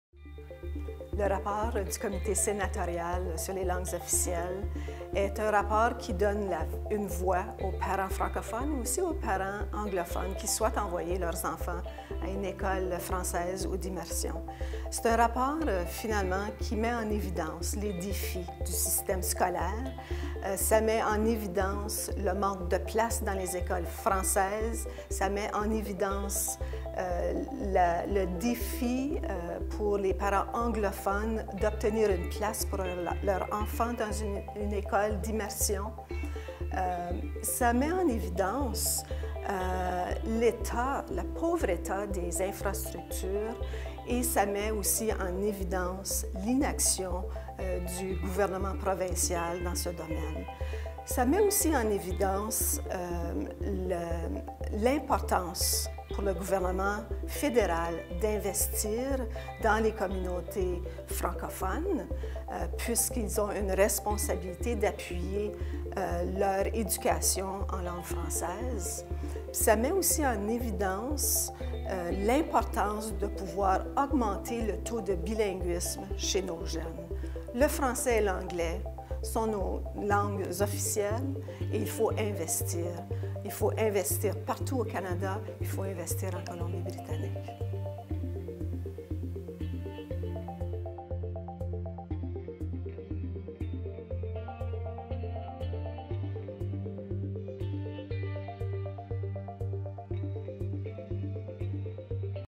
Un comité sénatorial pousse pour un meilleur accès à l’éducation en français en Colombie-Britannique
Dans son rapport intitulé Horizon 2018 : Vers un appui renforcé à l’éducation en français en Colombie-Britannique, le Comité sénatorial des langues officielles formule 17 recommandations pour permettre au gouvernement fédéral de respecter ses engagements en matière de langues officielles en vertu de la Charte canadienne des droits et libertés et de la Loi sur les langues officielles.